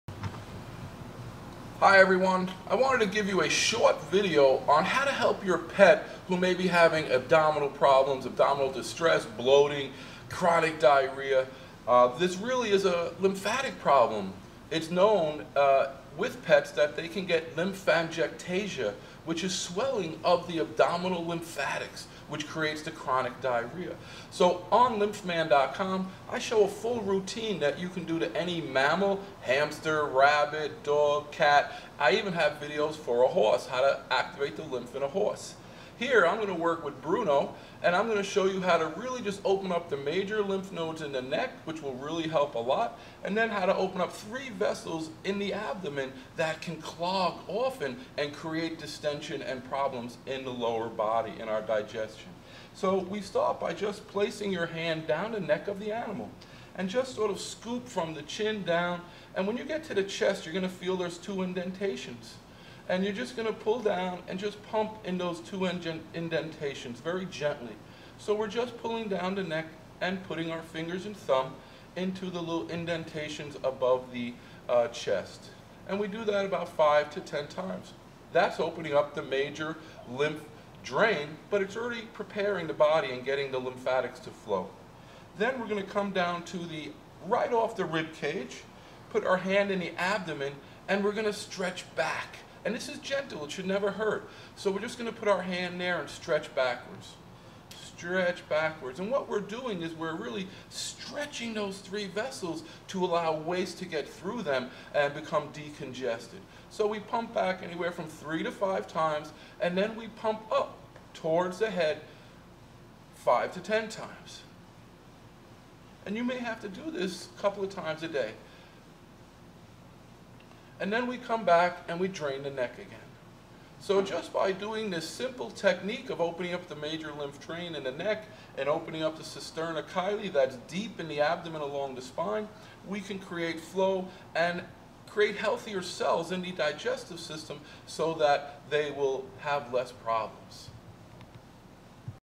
Lymphangiectasia - Opening Pets Cisterna Chyli
Help your pets Lymphangiectasia by opening their lymphatic system, specifically their cisterna chyli. John Ossipinsky explains how to help chronic diarrhea and digestive issues in animals by improving their lymph flow.